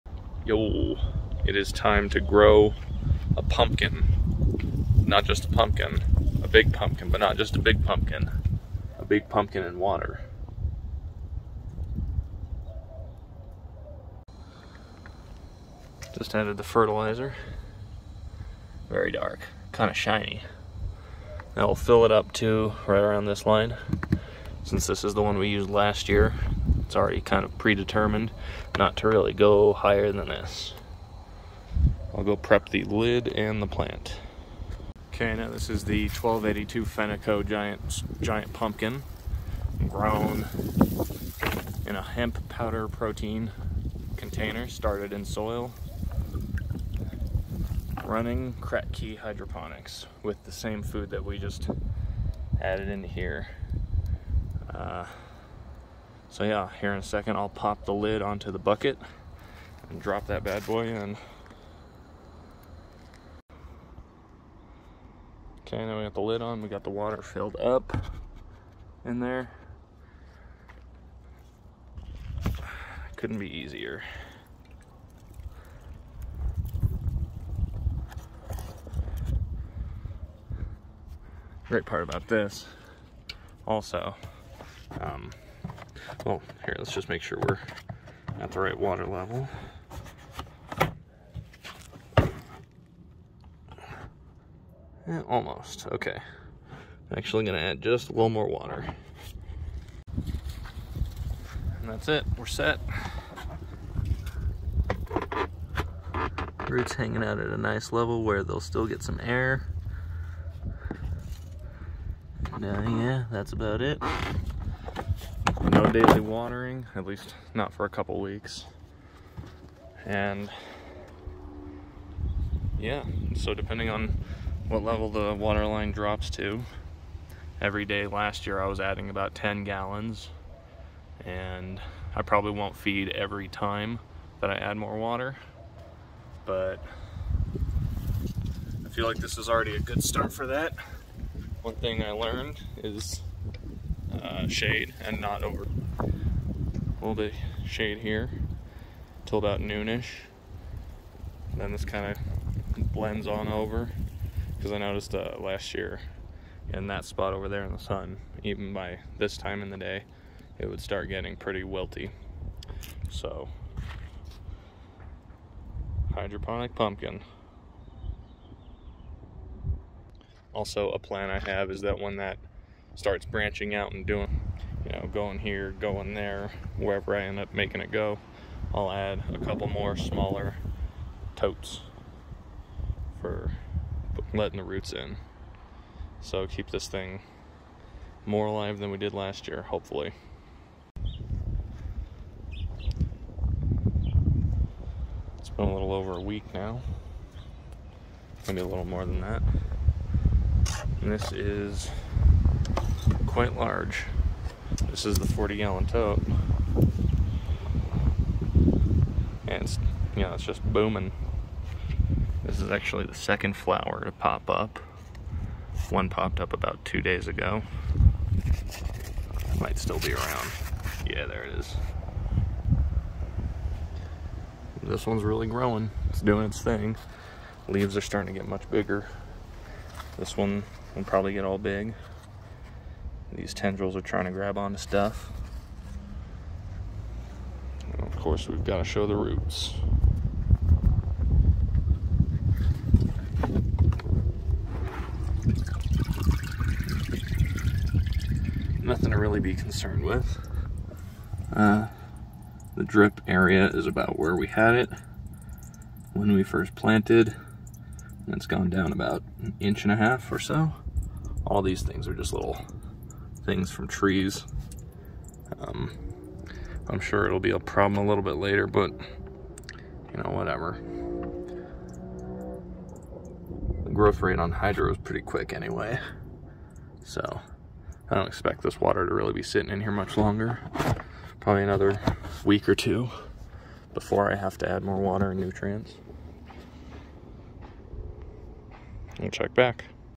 Kratky Hydroponic Giant Pumpkin Growing
Part 1 showing how I am growing a Giant Pumpkin in a 40 gallon Kratky Hydroponic tote. Thanks to Niles Fennikoh for the 1282 Seed!